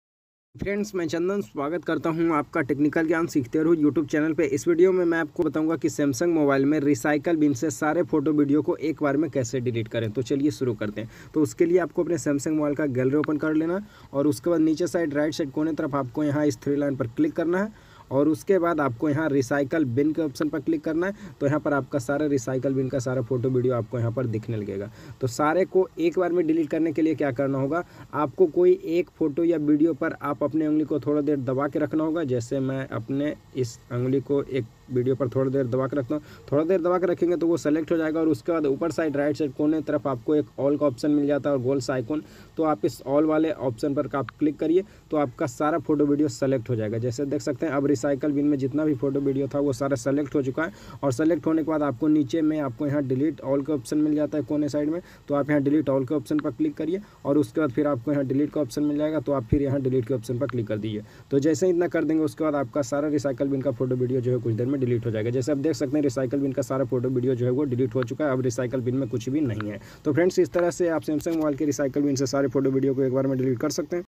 Samsung mobile ke recycle bin se Sare photo video ko ek bar Mein Kaise delete Karen
Hi friends is video mein main aapko Samsung mobile ke recycle bin se Sare photo video ko ek bar mein delete Karne Ka Tarika bataya hun

 to friends FIR milenge new video mein tab tak ke liye bye Radhe Radhe bolna padega